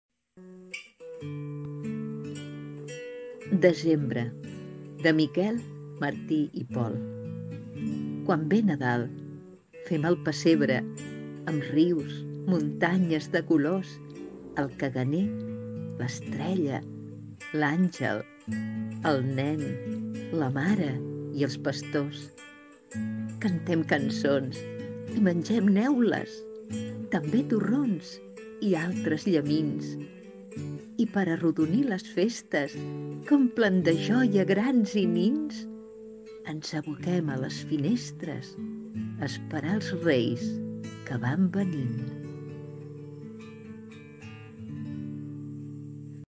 "Desembre" de Miquel Martí i Pol
Poema de  Miquel Martí i Pol
Rapsoda: María Dolors Quintana González
Guitarra: Eva Fraga González
Vídeo: Pere Líndez